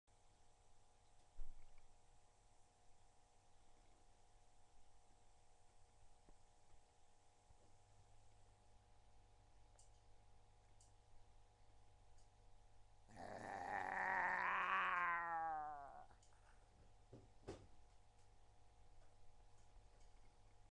Connor Pee'd Off
Connor the Cat is too dignified for this sort of thing at the grand old age of 15!